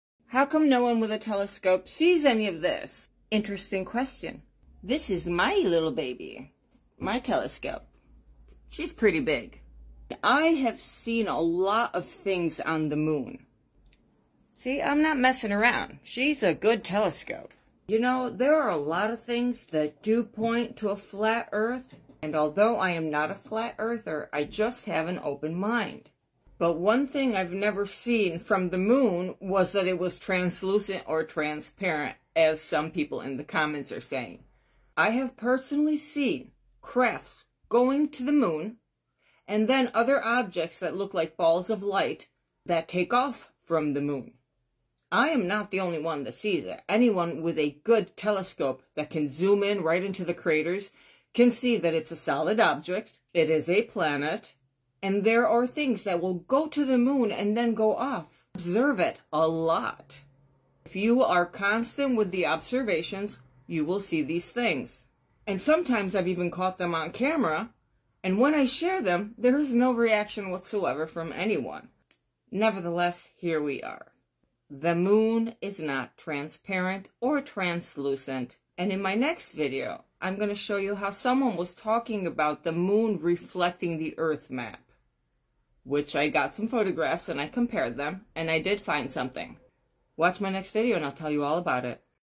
What I saw on the moon. How to view crafts and ufo uaps all around the moon
get a telescope and see for yourself the moon has so much going on. ufos and uaps all over